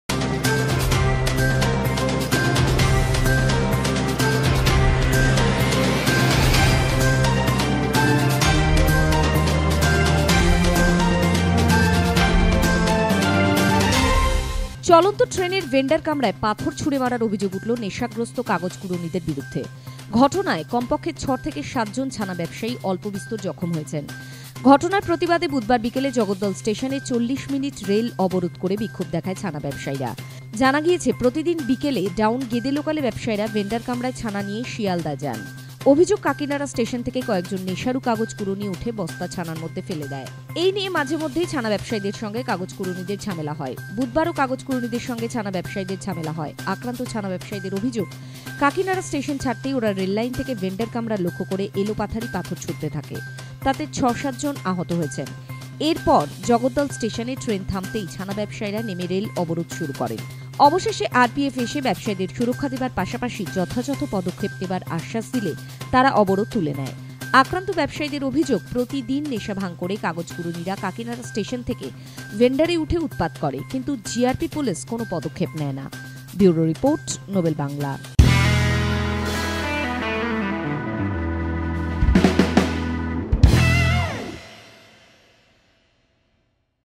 চলন্ত ট্রেনে পাথর, জখম ৭, Nobel Bangla News
চলন্ত ট্রেনে পাথর, জখম ৭, Nobel Bangla is Popular Bengali news channel, Nobel Bangla has now been branded